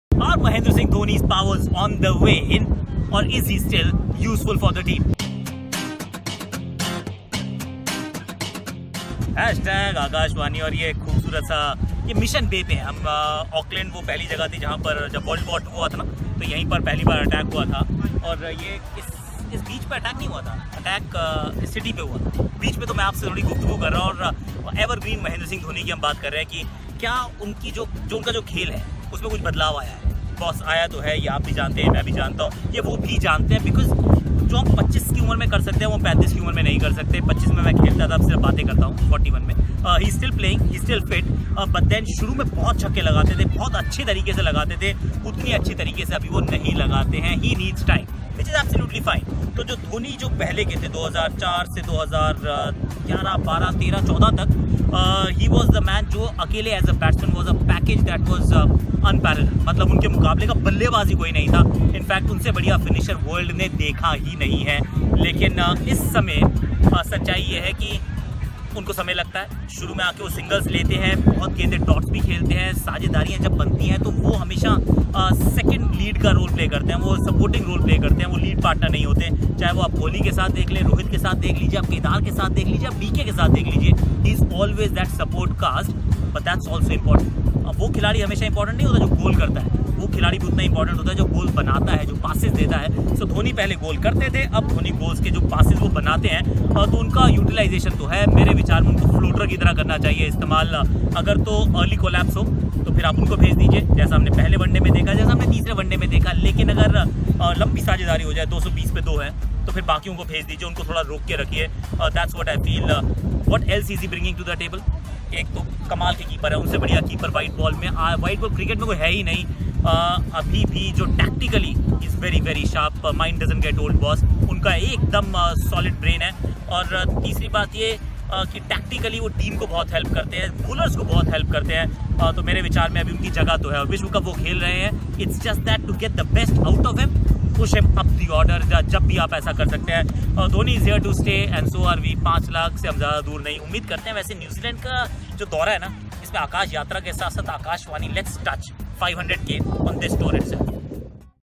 The MS DHONI Question
Three consecutive 50s, two great finishes, and a Man of the Series award in the AUSvIND ODIs - Is this a new MS Dhoni? How has he changed over the years? We talk about one of cricket's best finishers and how he's evolved over the years. Come on in, let's discuss the Dhoni question.